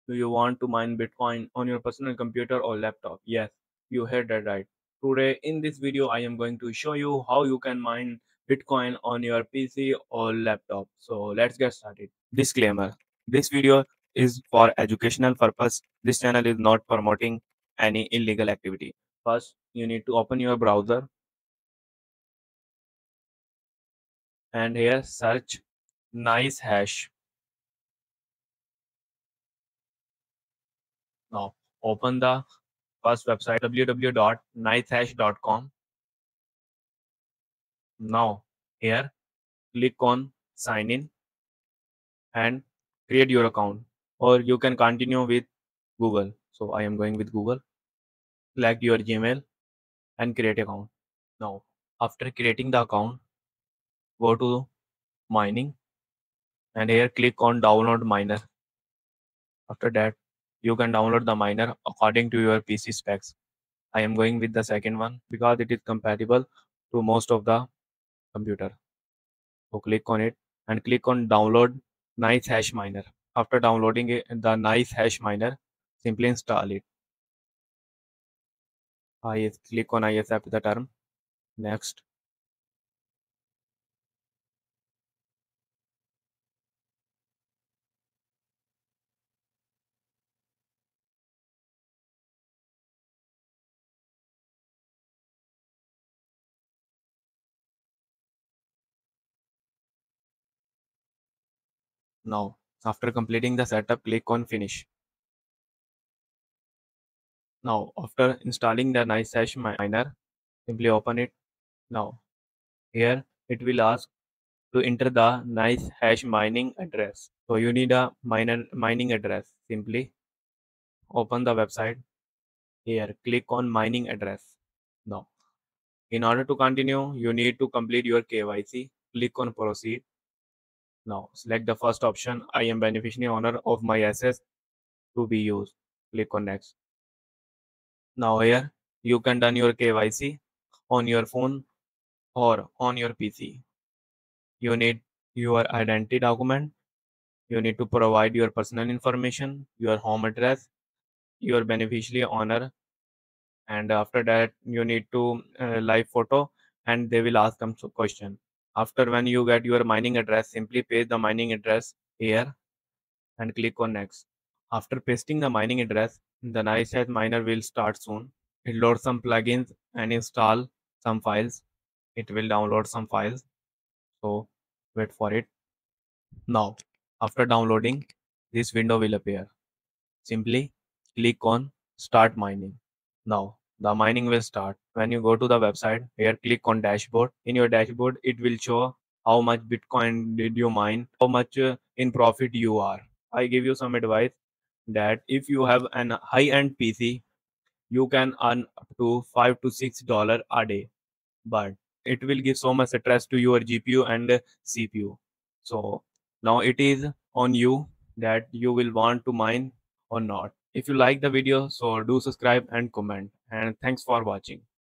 HOW TO MINE BITCOIN ON ANY LAPTOP & PC IN 2025 | Crypto Mining Tutorial | Nicehash ASIC Miner | 3Min
Get ready to unlock the secrets of Bitcoin mining in 2025! 📈💻 In this video, I'll show you *HOW TO MINE BITCOIN ON ANY LAPTOP & PC*, even if you're a beginner! 🤔💡 From setting up your rig to maximizing your profits, I've got you covered. Don't miss out on this opportunity to start earning Bitcoin from the comfort of your own home! 🏠💸